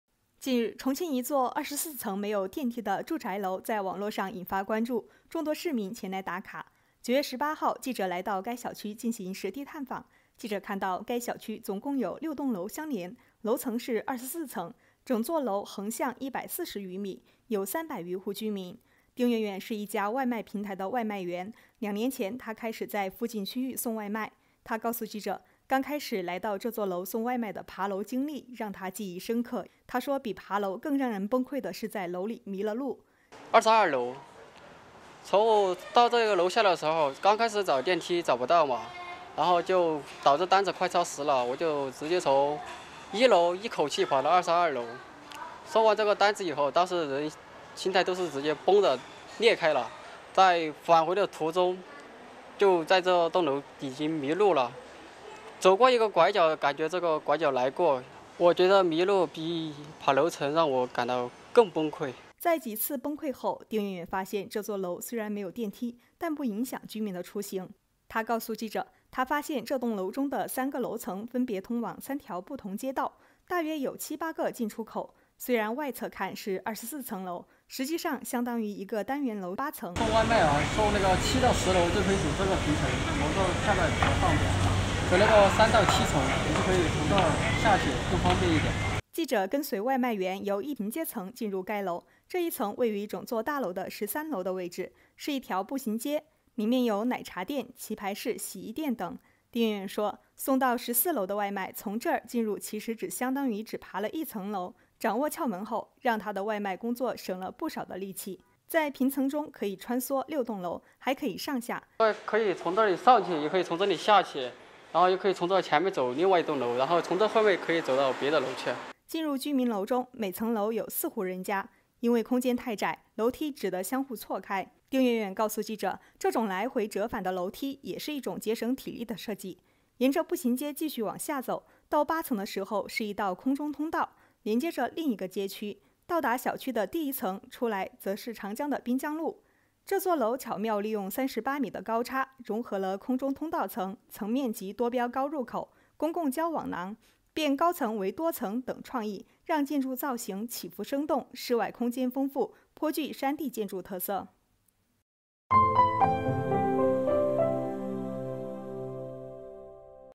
重庆一座24层楼没电梯 外卖员：曾在楼里迷路 心态崩了
近日，重庆一座24层没有电梯的住宅楼在网络上引发关注，众多市民前来“打卡”。9月18日，记者来到该小区进行实地探访。记者看到，该小区总共有6栋楼相连，楼层是24层，整座楼横向140余米，有300余户居民。

丁园园是一家外卖平台的外卖员，2年前他开始在附近区域送外卖。他告诉记者，刚开始来这座楼送外卖的爬楼经历让他记忆深刻。他说，比爬楼更让人崩溃的是在楼里迷了路。

在几次崩溃后，丁园园发现这座楼虽然没有电梯，但不影响居民的出行。他告诉记者，他发现这栋楼中的3个楼层分别通往3条不同街道，大约有7-8个进出口。虽然外侧看是24层楼，实际上相当于一个单元楼八层。

记者跟随外卖员由一平街层进入该楼，这一层位于整座大楼的13楼的位置，是一条步行街，里面有奶茶店、棋牌室、洗衣店等。丁园园说，送到14楼的外卖，从这儿进入其实只相当于只爬一层楼，掌握“窍门”后，让他的外卖工作省了不少的力气。在平层中可以穿梭6栋楼，还可以上下。

进入居民楼中，每层楼有4户人家，因为空间太窄，楼梯只得相互错开。丁园园告诉记者，这种来回折返的楼梯也是一种节省体力的设计。

这座楼巧妙利用38米的高差，融合了“空中通道层”、“层面及多标高入口”、“公共交往廊”、“变高层为多层”等创意，让建筑造型起伏生动，室外空间丰富，颇具山地建筑特色。（贾楠 生薛健）